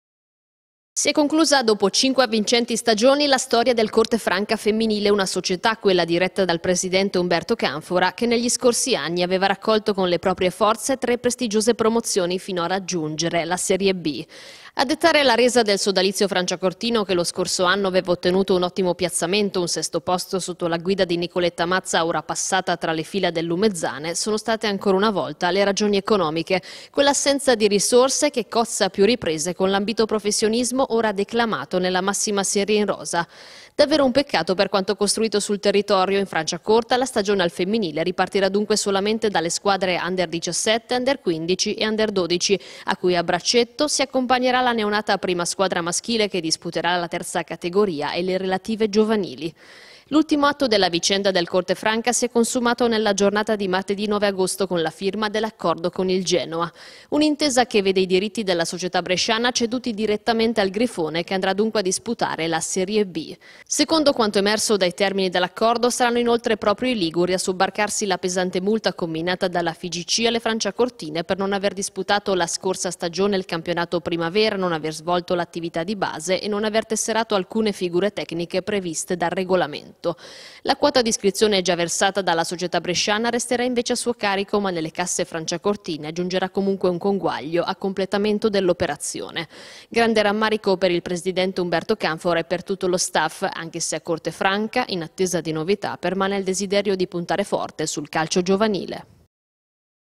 CORTEFRANCA AL CAPOLINEA, TITOLO AL GENOA
CORTEFRANCA NEL BUIO: PER PROBLEMI ECONOMICI LA SOCIETÀ DEL PRESIDENTE CANFORA SI È VISTA COSTRETTA A CEDERE IL TITOLO DI SERIE B FEMMINILE AL GENOA -
Cortefranca nel buio. A causa della mancanza di risorse economiche la società del presidente Canfora si è vista costretta a cedere il titolo di Serie B femminile al Genoa. Ripartirà dalle giovanili e dalla prima squadra maschile.